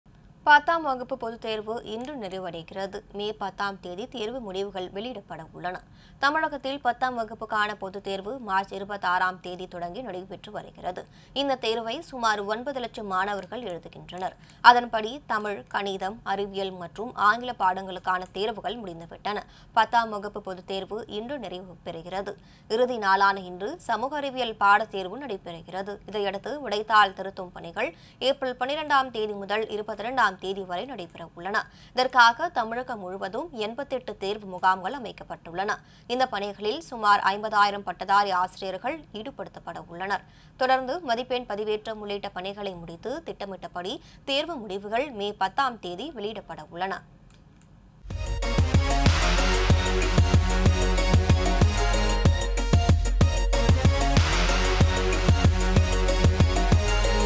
பத்தாம் வகுப்புப் பொதுத்தோ்வு இன்று நிறைவு: மே 10ஆம் தேதி தோ்வு முடிவுகள் வெளியீடு | Public Exam
பத்தாம் வகுப்புப் பொதுத்தோ்வு இன்று நிறைவடைகிறது. மே 10ஆம் தேதி தோ்வு முடிவுகள் வெளியிடப்படவுள்ளன.